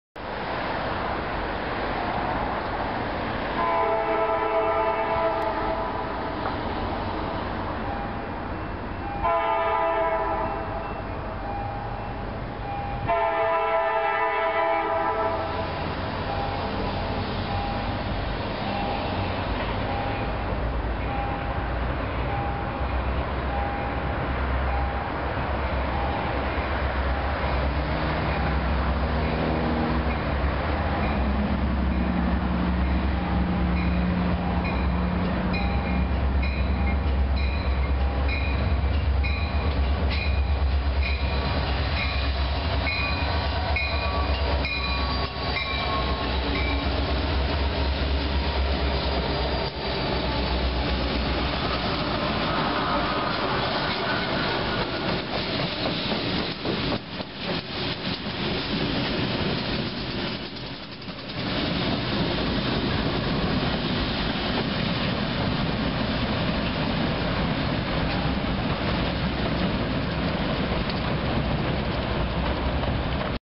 CSX Power Moving West at S. Framingham
CSX Power moving west at South Framingham MA on 5/16/2010.